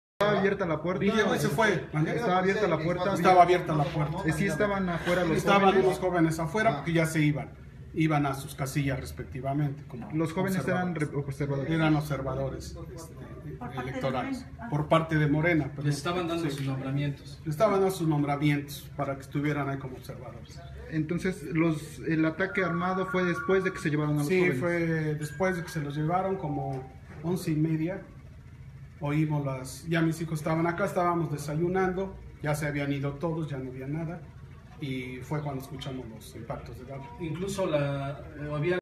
Balean domicilio en San Manuel tras detención de simpatizantes de Morena
Un grupo armado atacó a balazos un domicilio de la colonia San Manuel minutos después de que 15 simpatizantes de Morena fueron detenidos con violencia y sin orden judicial por policías municipales, que los acusaron de delitos electorales.